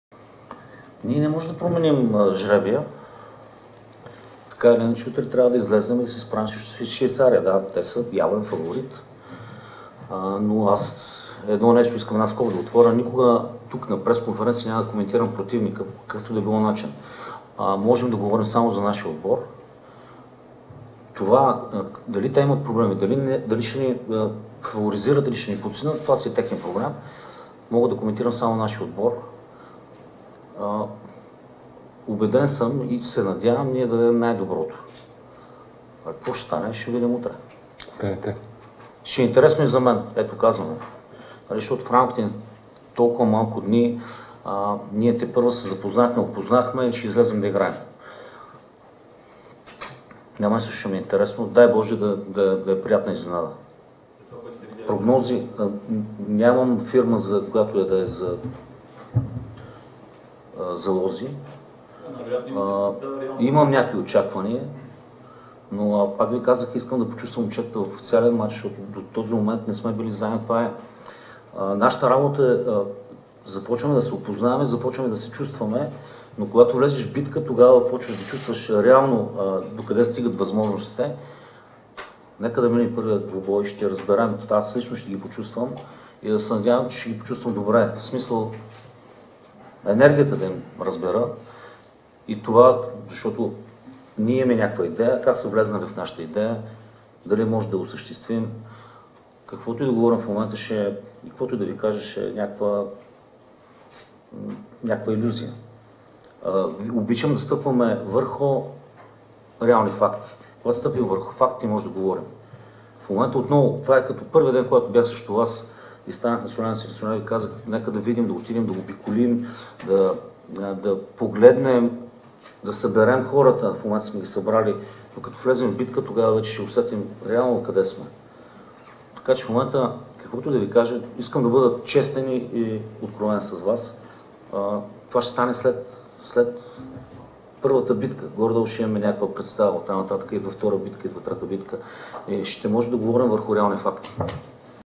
Ясен Петров: Ще видим реално докъде сме стигнали, когато влезем в битка
Селекционерът на България Ясен Петров даде пресконференция преди утрешния мач на представителния ни тим с Швейцария. "Лъвовете" приемат своя противник на националния стадион "Васил Левски" утре, 19:00 часа в световната квалификация. Днешният брифинг беше излъчен НА ЖИВО в сайта Sportal.bg и по Sportal TV. Петров обяви кой ще носи капитанската лента на отбора ни, както и какво е състоянието на Андрей Гълъбинов.